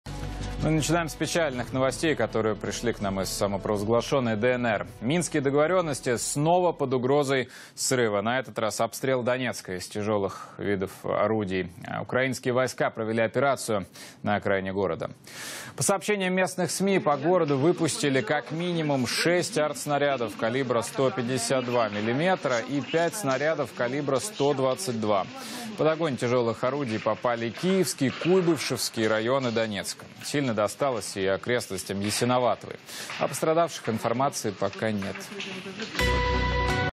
Украинская армия начала обстрел окраин Донецка из тяжелых орудий
Выпущено минимум 11 снарядов крупных калибров